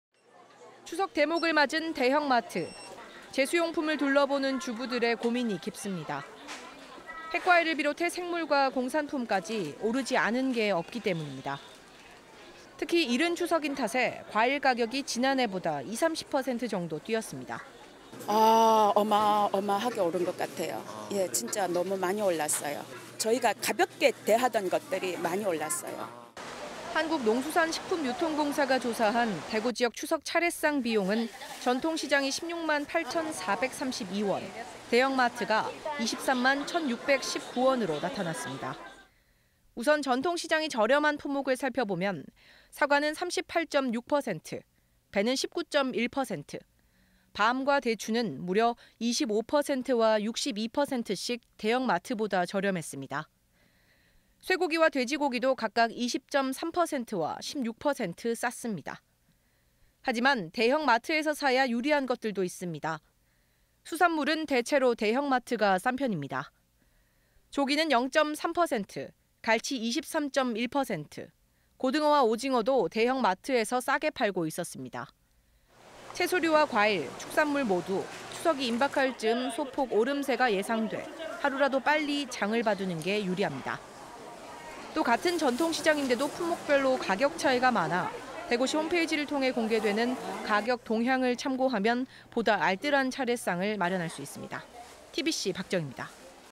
추석 장보기 어디가 저렴할까? 190909 [TBC-띠비띠]
이른 추석, 과일 가격이 지난해보다 20~30% 올랐습니다